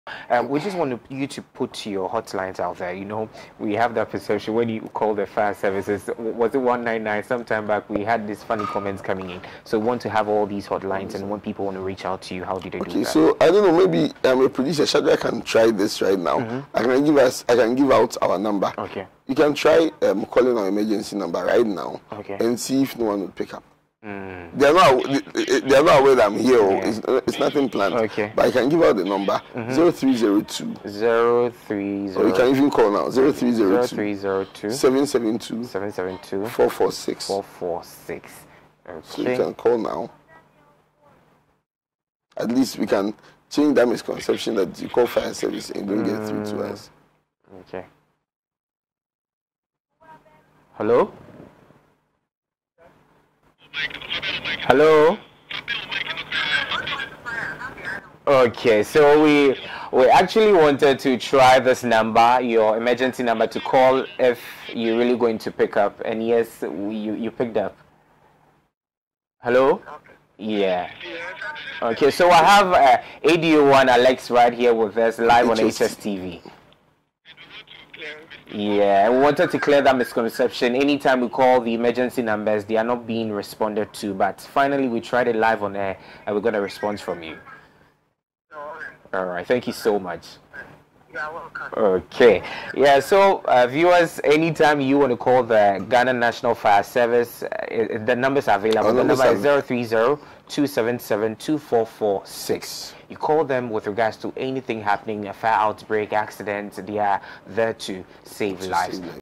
HSTV calls Ghana National Fire Service emergency line live on air.Check what happened
HSTV calls Ghana National Fire Service   emergency line live on air.